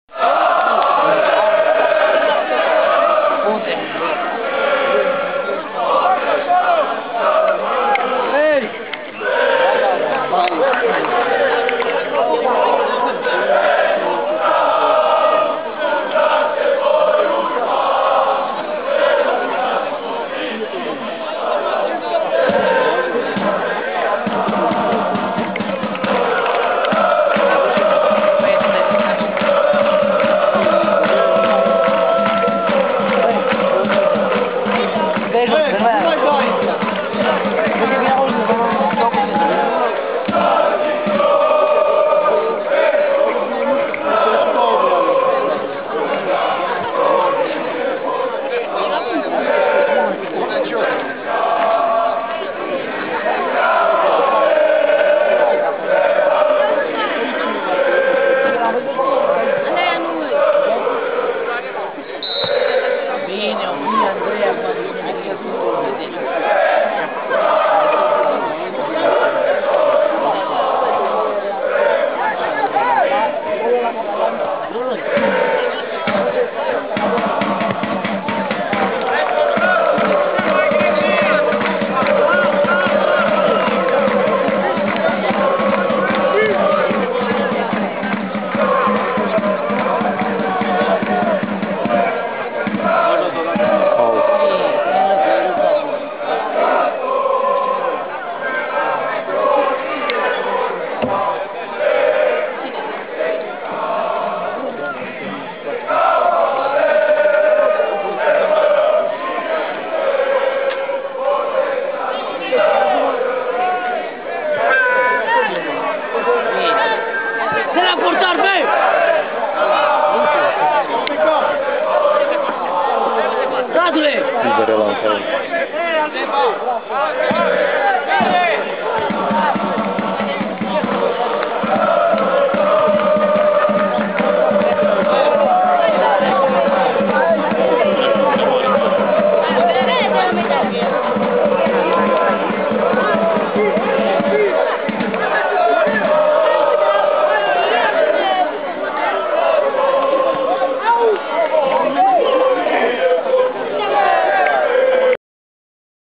Leucuşeşti - ASU POLI 1-4 Inimi de cavaleri
Unirea Leucuşeşti - ASU POLI 1-4 prima deplasare in judeţ a Peluzei Sud. Liga 5 Romania.